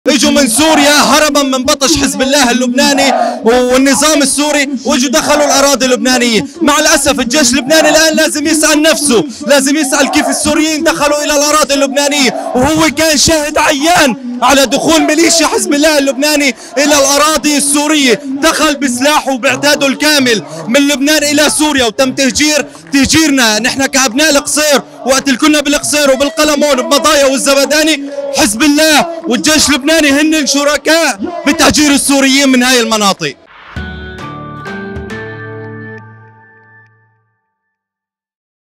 مظاهرة ضد الترحيل القسري للاجئين السوريين من لبنان
نظم ناشطون في محافظة إدلب شمال غرب سوريا تظاهرة للتنديد بعمليات الترحيل القسري التي تمارسها الحكومة اللبنانية بحق اللاجئين السوريين في لبنان.